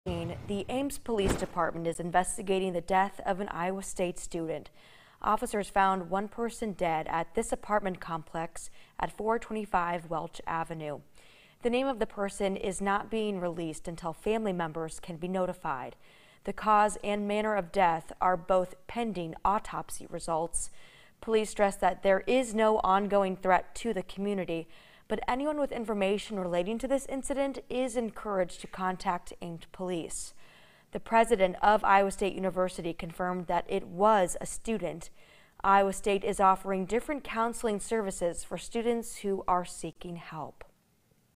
Ames Police investigating death of Iowa State University student
The Ames Police Department is investigating the death of an Iowa State University student.